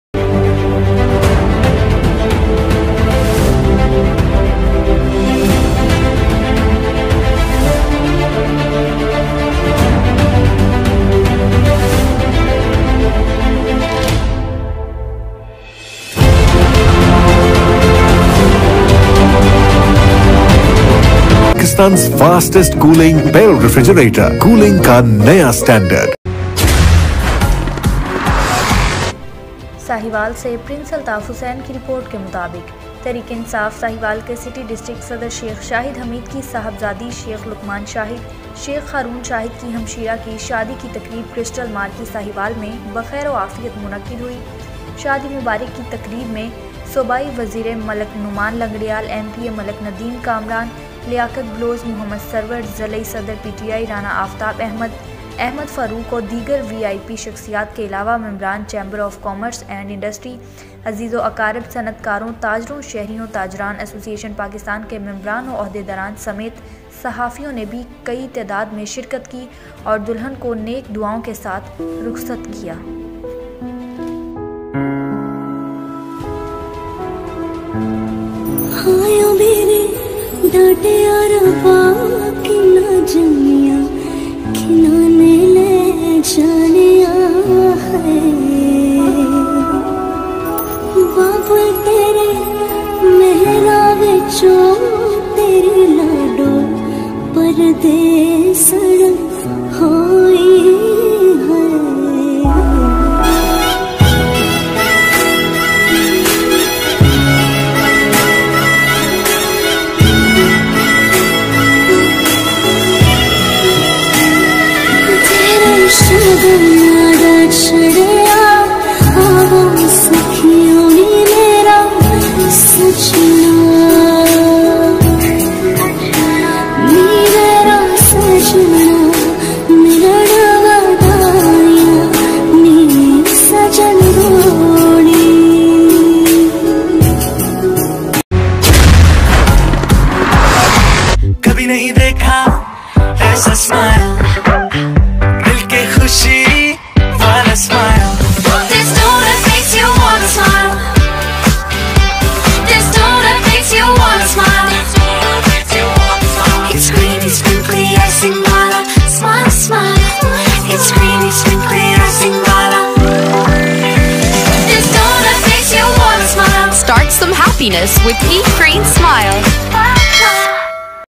چیف ڈائریکٹر پرنس الطاف حسین/ANY NEWS HD/
تحریک ا نصاف ساہیوال کے سٹی ڈسٹرکٹ صدر شیخ شاہد حمید کی صاحبزادی ،شیخ لقمان شاہد ،شیخ ہارون شاہد کی ہمشیرہ کی شادی کی تقریب کرسٹل مارکی ساہیوال میں الحمداﷲ بخیر و عافیت منعقد ہوئی ۔ شادی مبارک کی تقریب میں صوبائی وزیر ملک نعمان لنگڑیال ،ایم پی اے ملک ندیم کامران ، سابق ضلعی ناظم رائے حسن نواز خاں، جماعت اسلامی کے جنرل سیکرٹری لیاقت بلوچ، صدر چیمبر آف کامرس شےخ بشارت ندیم، مرکزی رہنما پی ٹی آئی میجر (ر) محمد سرور ۔ ضلعی صدر پی ٹی آئی رانا آفتاب احمد خاں،سابق ایم پی اے شیخ محمد چوہان ۔ سابق صدر چیمبر آف کامرس شیخ شہزاد احمد فاروق ودیگر وی وی آئی پی شخصیات کے علاوہ ممبران چیمبر آف کامرس اینڈ انڈسٹری ،عزیز و اقار ت ،صنعتکاروں،تاجروں ، شہریوں ،تاجران ایسوسی ایشن پاکستان کے ممبران و عہدیداران سمیت صحافیوں نے بھی کثےر تعداد میں شرکت کی۔اور دلہن کو نیک دُعاﺅں کے ساتھ رخصت کیا۔ اس موقع پر شےخ شاہد حمید ،شیخ لقمان شاہد، شیخ ہارون شاہد(کوہ نور انڈسٹری) نے معز ز مہمانوں کو بھر پور ویلکم کیا۔